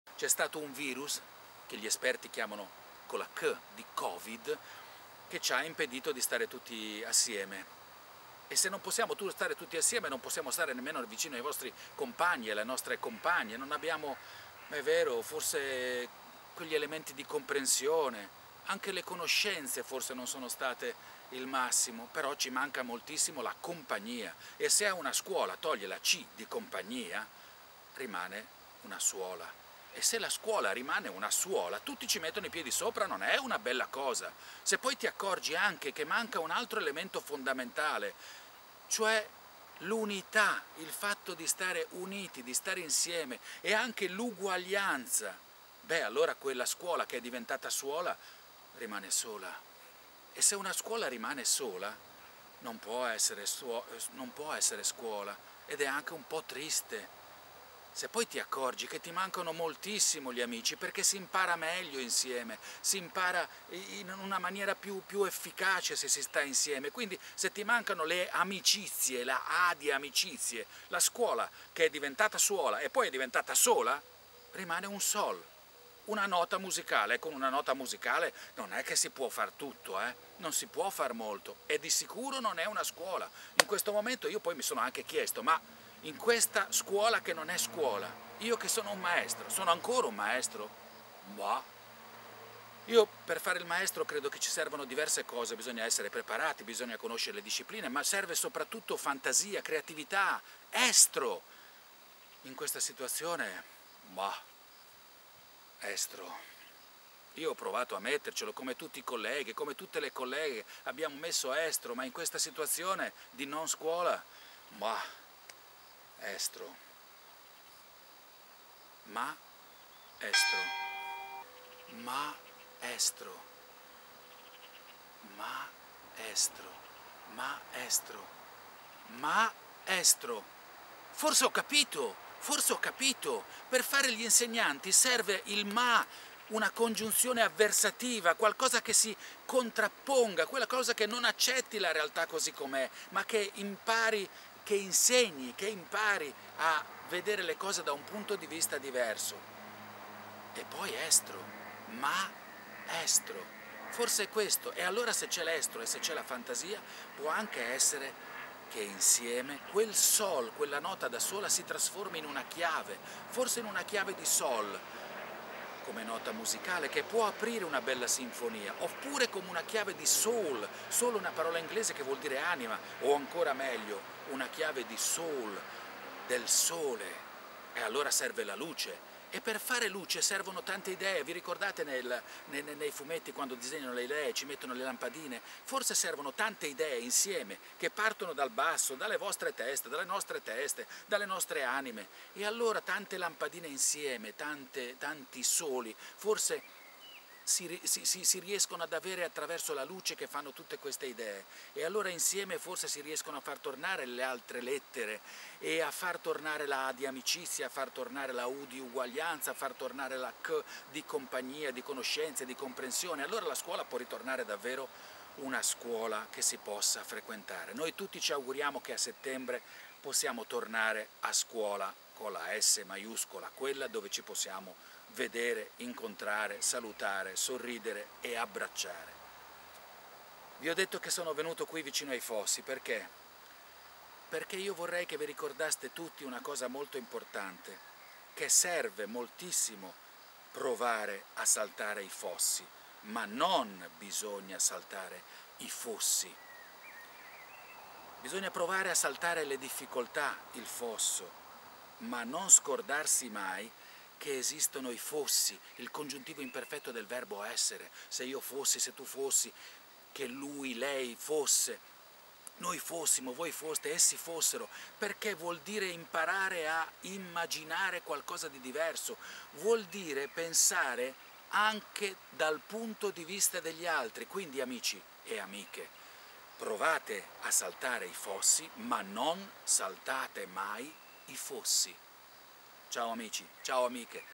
Scuola, suola, sola, sol, so, s... sssh
Un estratto preso dalla mia introduzione al filmato di saluto che i bambini e le bambine della classe quinta della scuola primaria "Bruno Ciari" hanno presentato ai loro compagni, ai genitori e al personale il 5 giugno 2020...nonostante non fossimo a scuola.
Il post del mio blog in cui è incorporato ed accompagnato dalle mie parole è qui